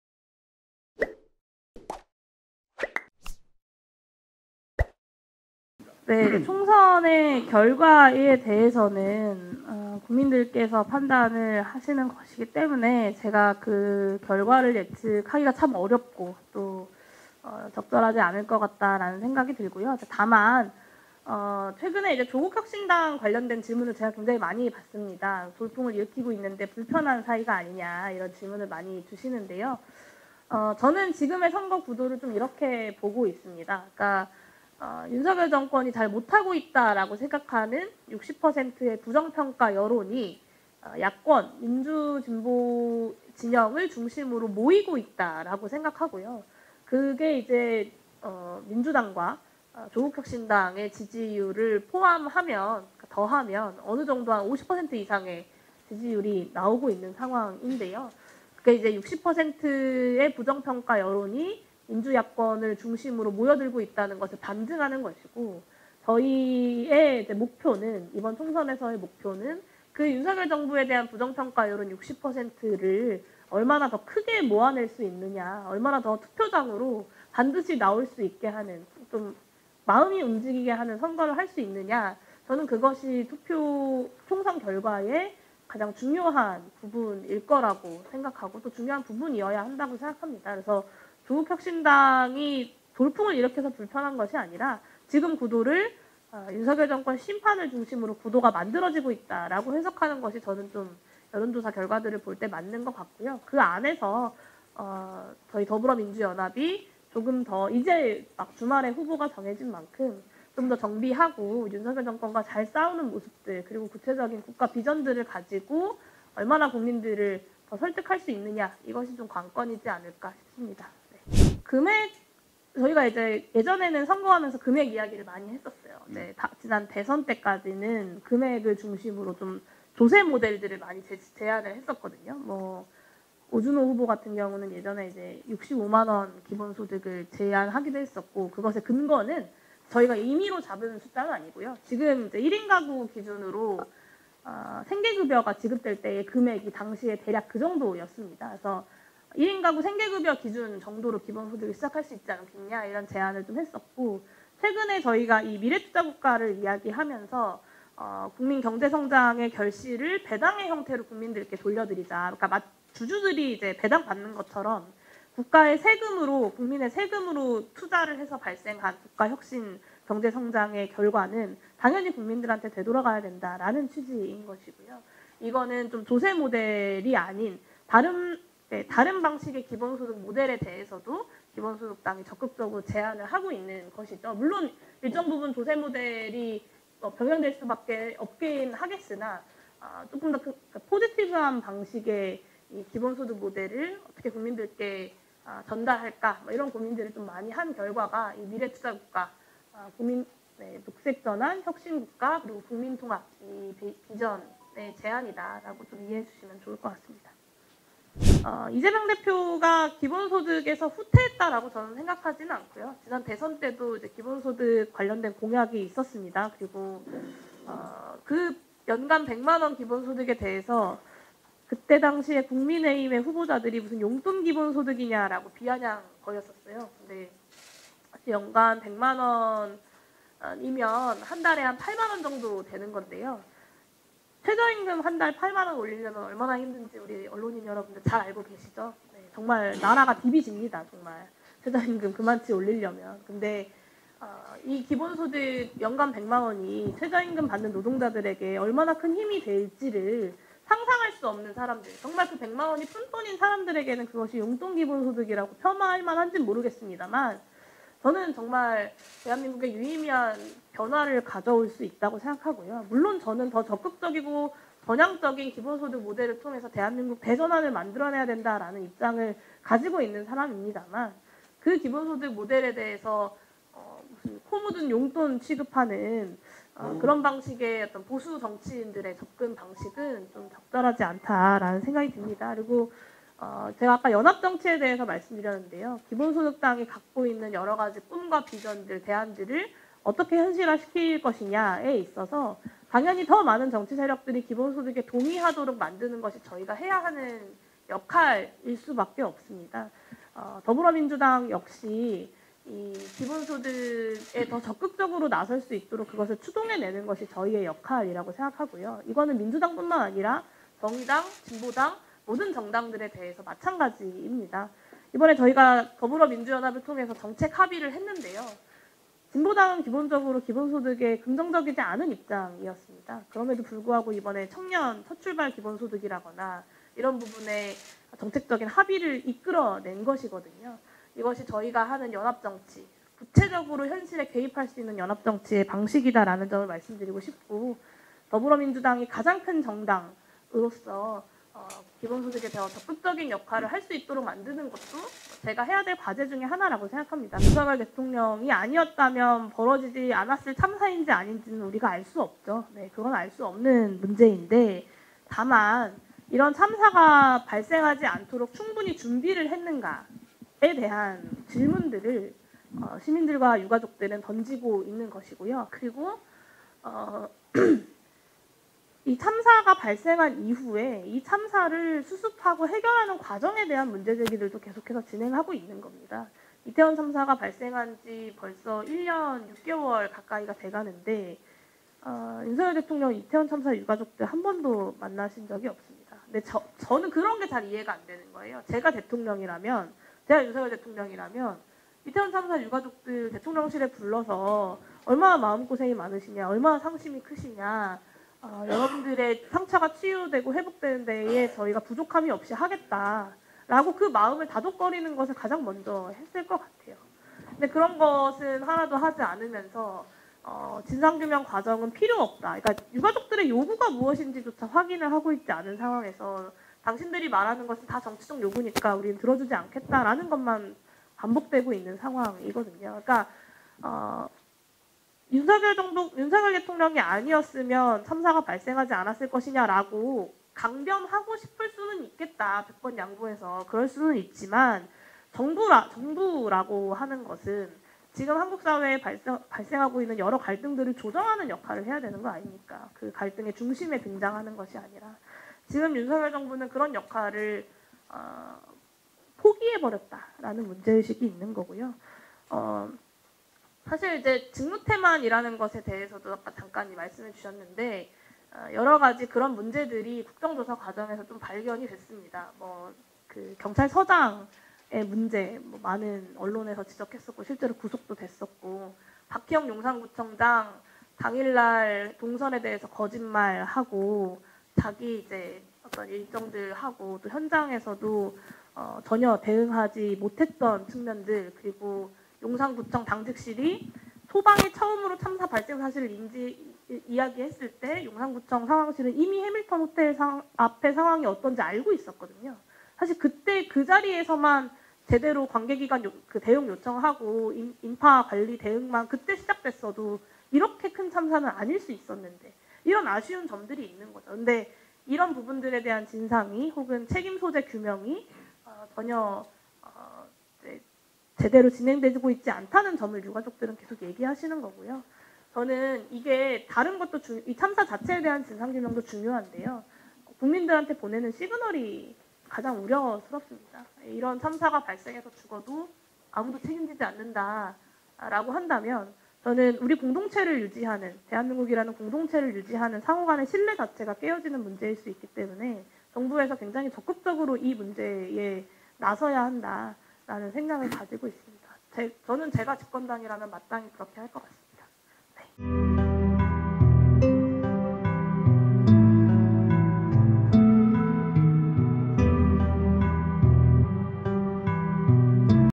용혜인 ② "이태원 참사 1년 6개월째···윤석열 정부는 역할 포기" | 뉴스플러스
기본소득당은 2020년 1월에 창당했습니다. 선별적 복지가 아닌 모든 국민에게 기본소득을 지급해야 한다는 정책을 내세웁니다. 이번 제22대 국회의원 선거를 앞두고 사회민주당, 열린민주당과 정당 연합을 제안해 새진보연합으로 이름을 바꿨고, 이후 더불어민주당, 진보당과 함께 더불어민주연합을 구성했습니다.

'당원 평균 나이 27세'의 젊은 정당인만큼 여러 이슈에서 기존 정당보다 진보적인 목소리를 내고 있는데요, 하지만 기본소득당의 유일한 국회의원인 용혜인 의원도 더불어시민당에 합류해 비례대표 후보로 당선되어야 했던 만큼 지역 기반이 약하고 따라서 다른 정치세력과의 연합에 몸을 실어야 하는 한계도 있습니다. 용혜인 더불어민주연합 의원은 3월 19일 대구에서 열린 '아시아 포럼 21' 정책토론회에 참석했는데요, 어떤 이야기를 했는지 자세히 들어봤습니다.